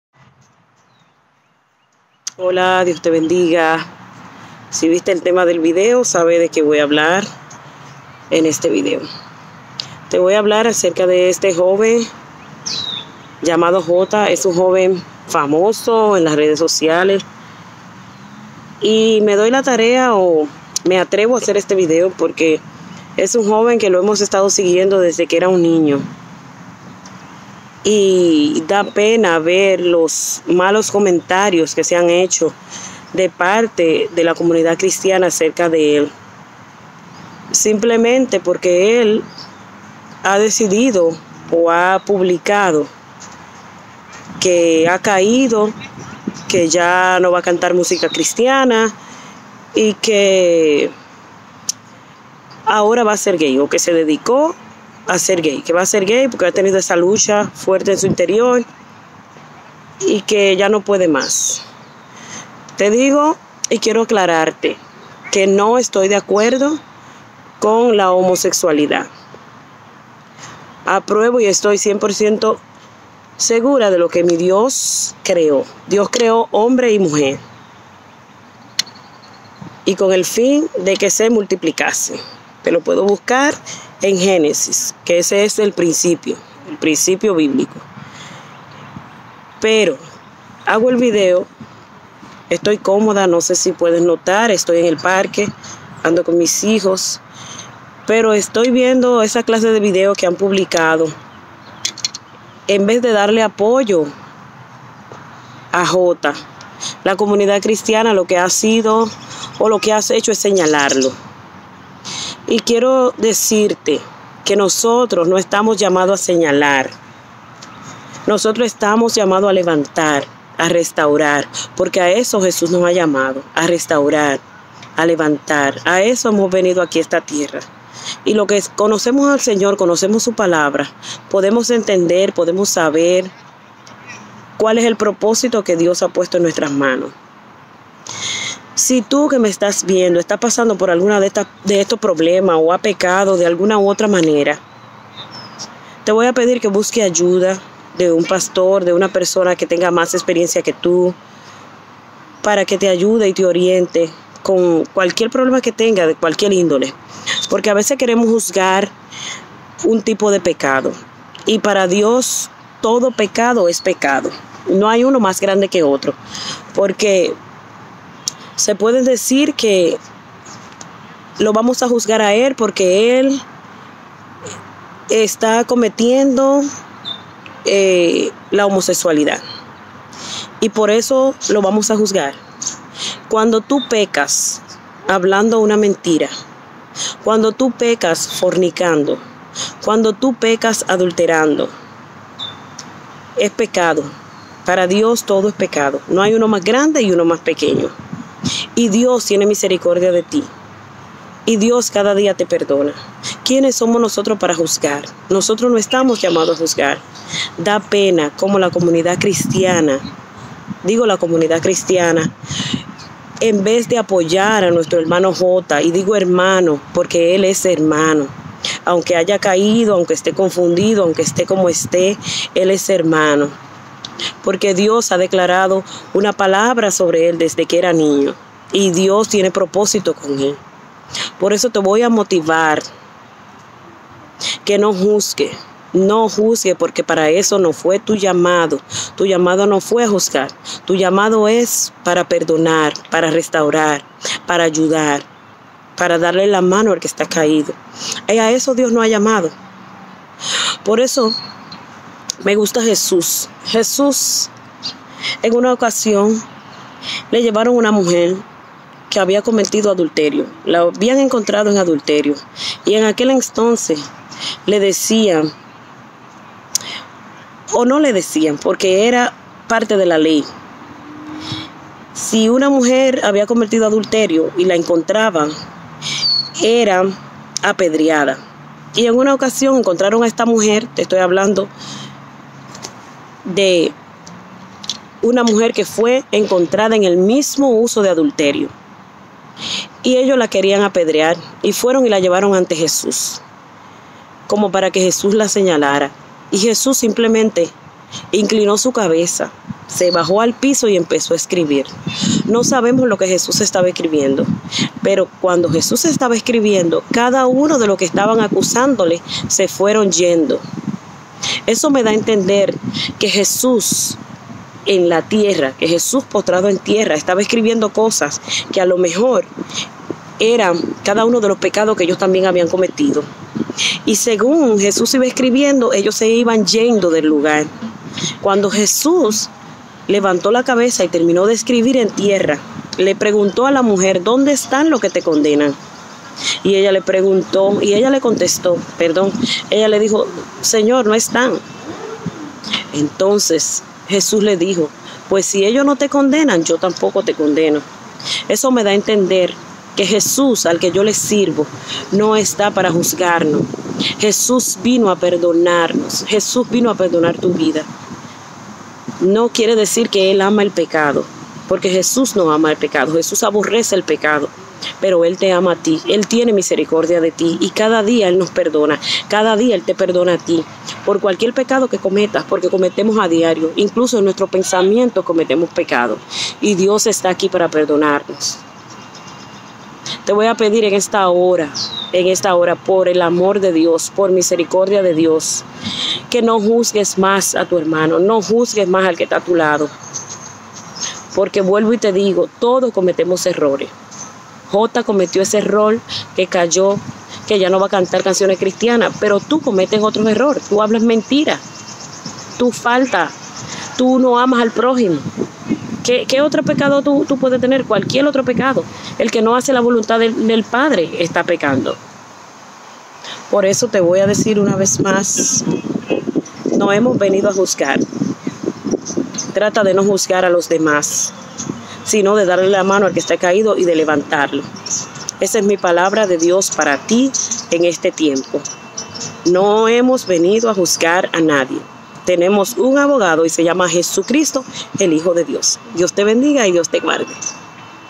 No juzgues. Mi opinión sobre Jotta A
Jotta A se declara homosexual y lo queremos crucificar.
¿Porque lo queremos Juzgar Tu y Yo?
Acaso. ¿SOMOS PERFECTOS?
DÉJALE ESO A DIOS 5/23/2020